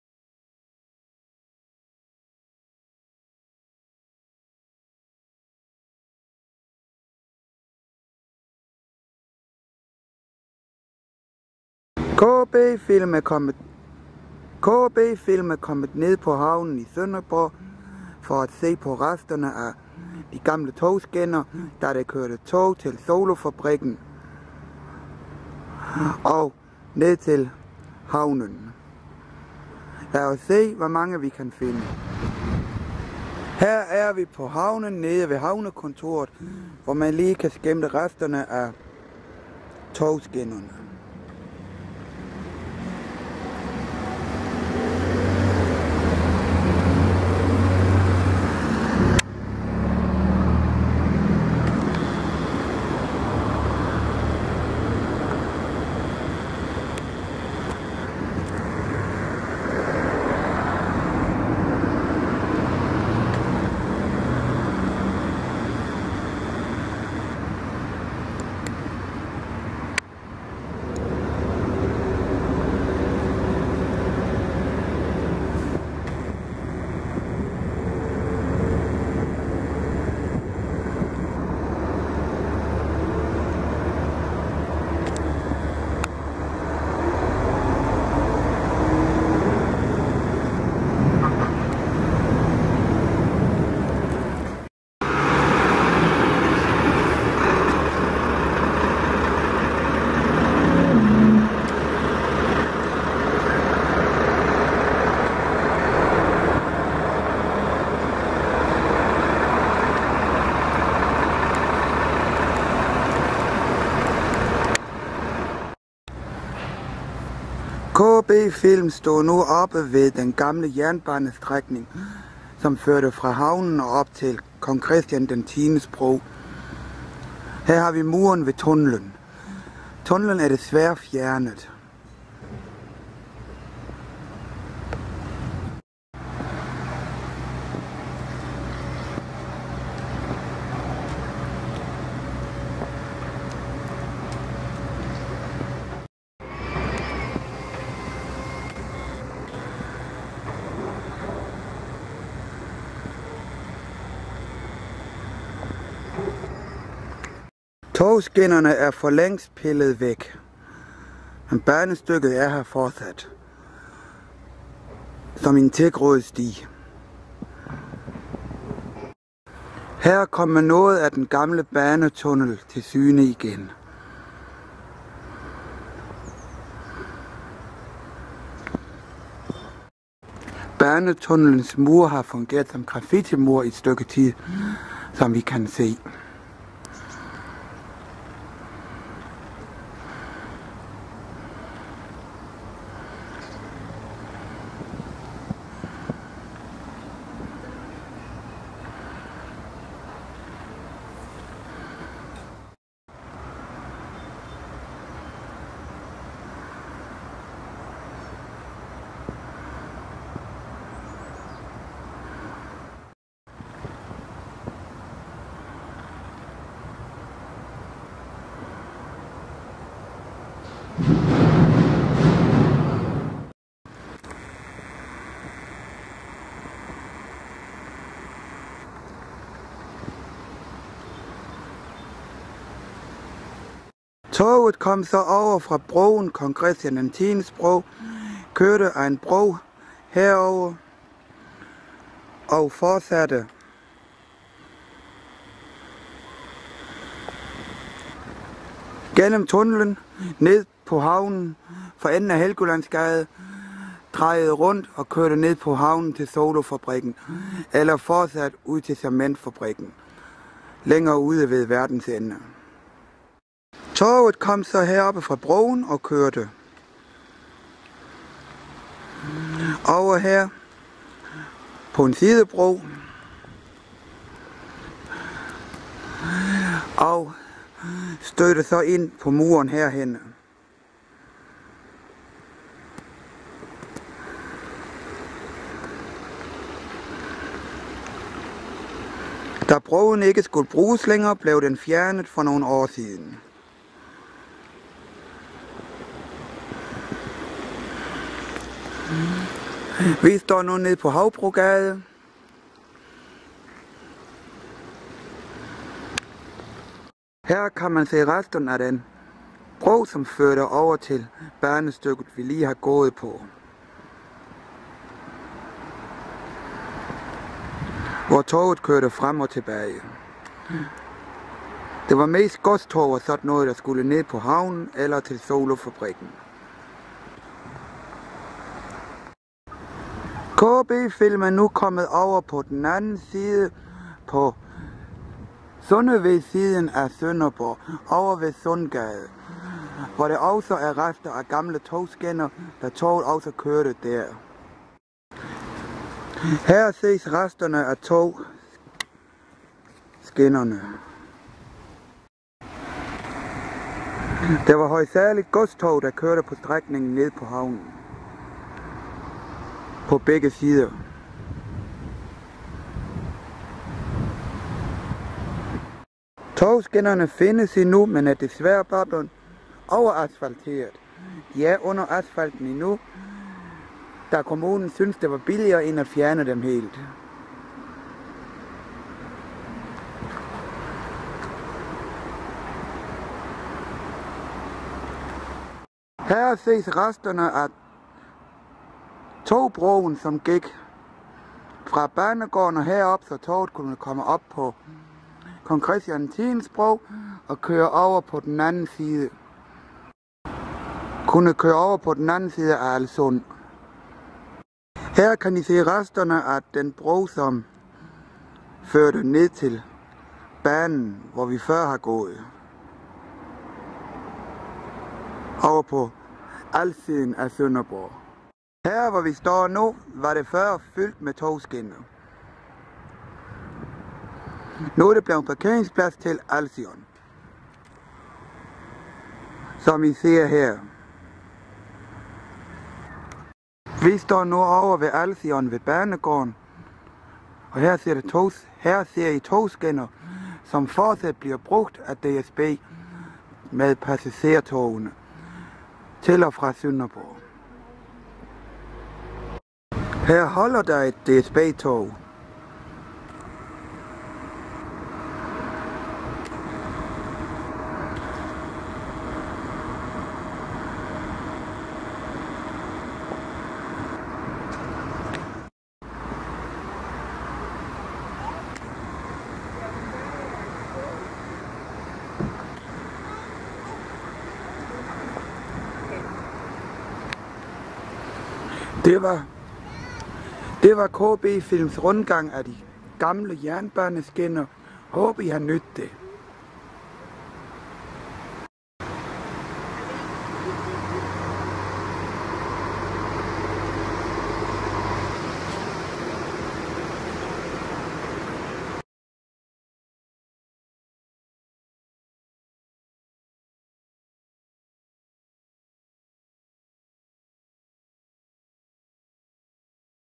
De gamle togskinner på Sønderborg havn (HD 1080p)
her kan du se rester af de gamle togskinner på Sønderborg havn